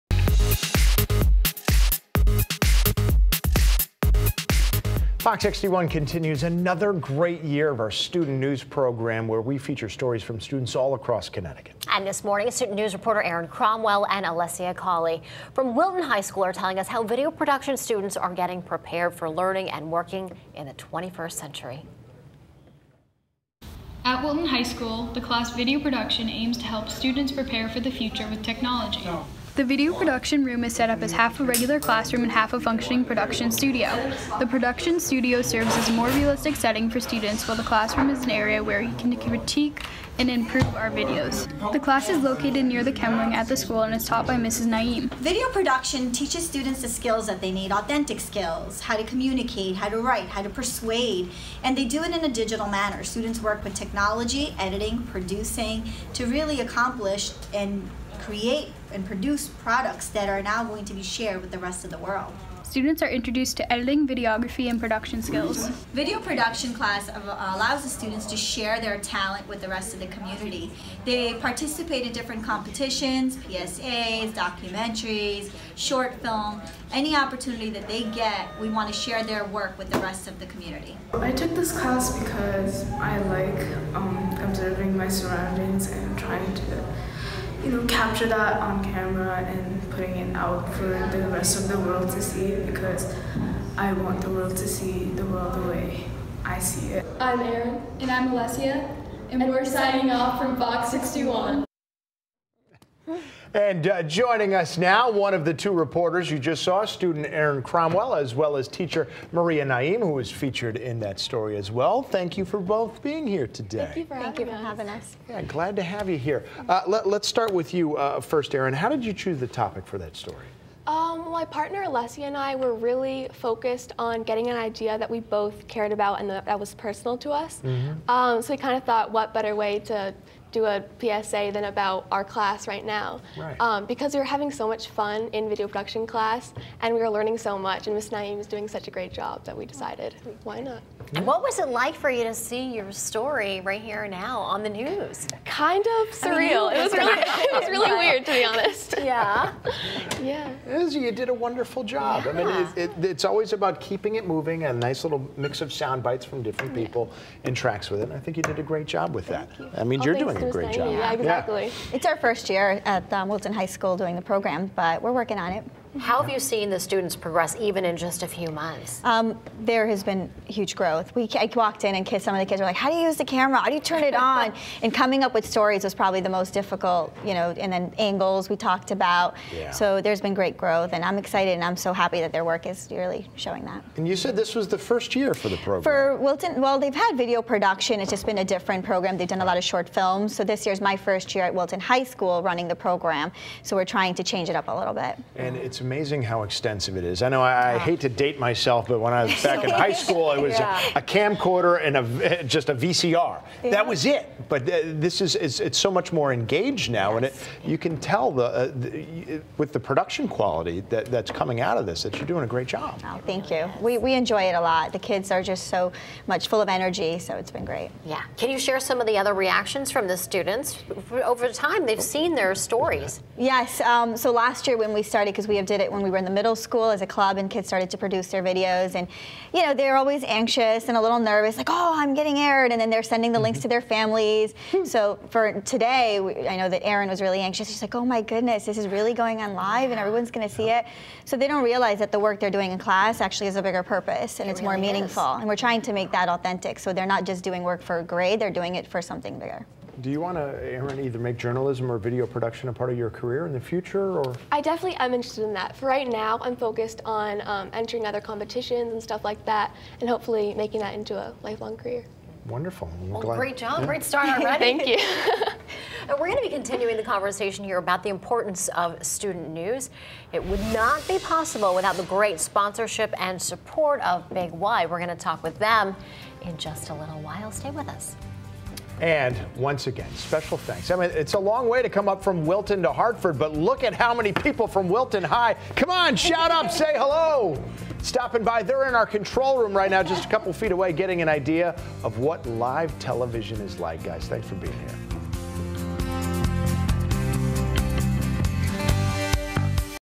wilton high school student news
FOX 61 CONTINUES ANOTHER YEAR OF OUR STUDENT NEWS PROGRAM..   WHERE WE FEATURE STORIES FROM STUDENTS ALL ACROSS CONNECTICUT.
   THIS MORNING-
    STUDENT NEWS REPORTERS ERIN CROMWELL AND ALESSIA CAWLEY FROM WILTON HIGH SCHOOL TELL US HOW VIDEO PRODUCTION STUDENTS ARE BEING PREPARED FOR LEARNING AND WORKING IN THE 21ST CENTURY .

JOINING US, STUDENT ERIN CROMWELL AND TEACHER MARIA NAEEM